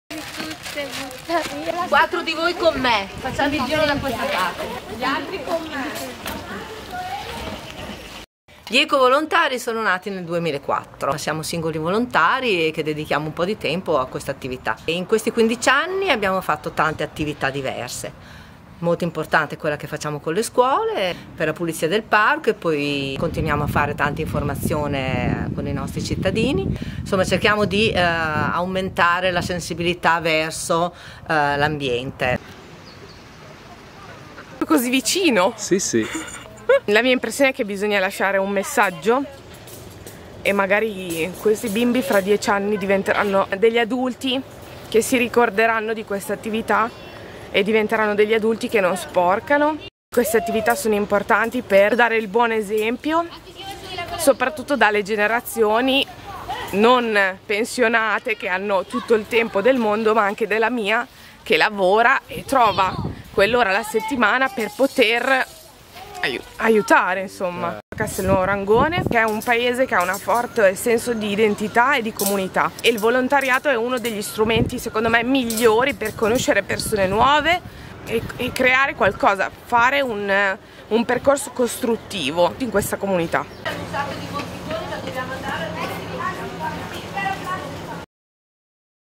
Gli ecovolontari di Castelnuovo Rangone
Attivi con le scuole e nelle feste di paese, accrescono la sensibilità ambientale e l'informazione sulla raccolta differenziata. Se desideri essere utile alla comunità, unisciti a loro!